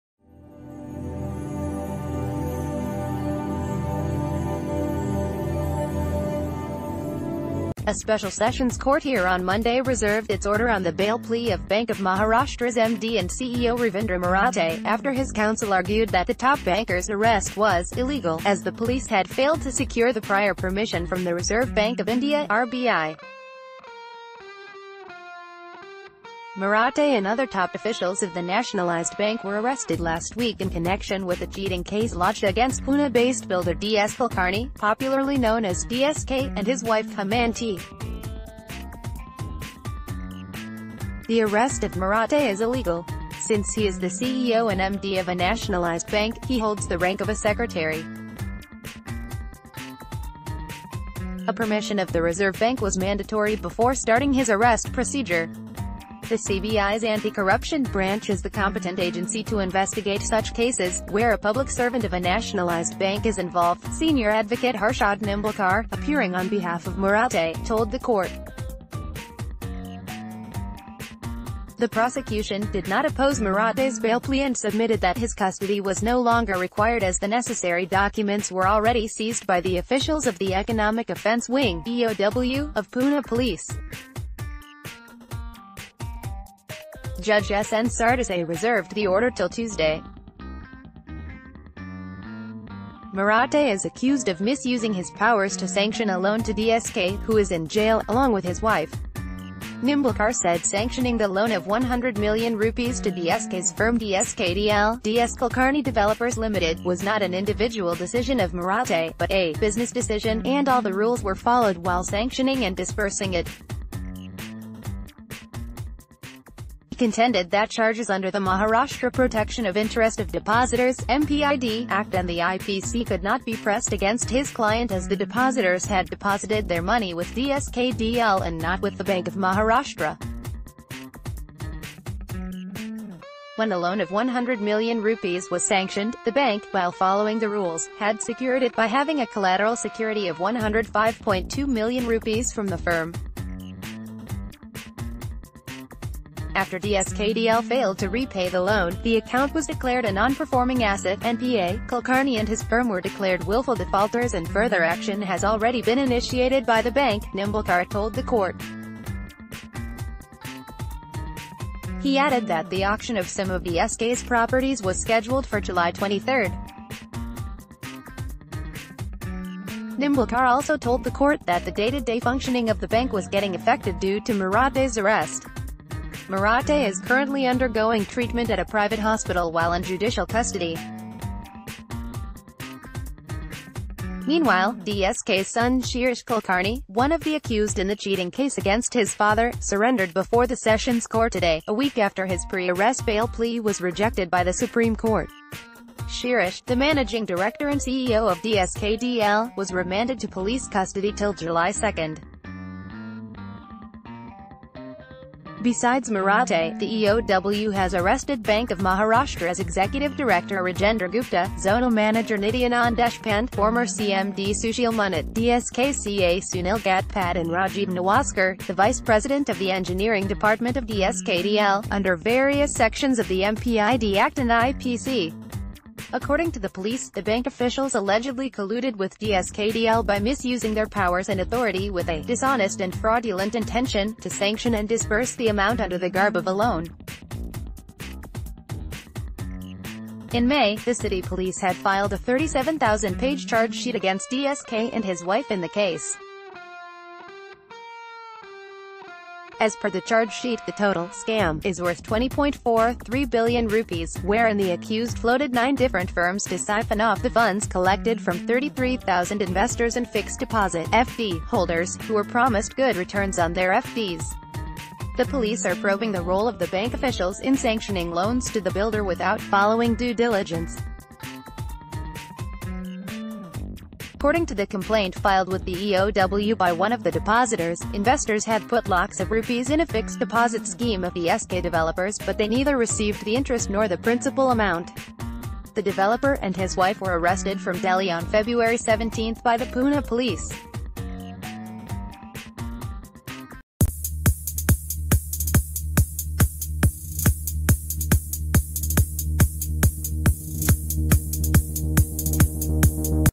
Breaking News | DSK Group scam: Pune court reserves bail plea of BoM's CEO Ravinder Marathe
A special sessions court here on Monday reserved its order on the bail plea of Bank of Maharashtra's MD and CEO Ravindra Marathe, after his counsel argued that the top banker's arrest was "illegal" as the police had failed to secure the prior permission from the Reserve Bank of India (RBI). Marathe and other top officials of the nationalised bank were arrested last week in connection with a cheating case lodged against Pune-based builder D S Kulkarni, popularly known as DSK, and his wife Hemanti...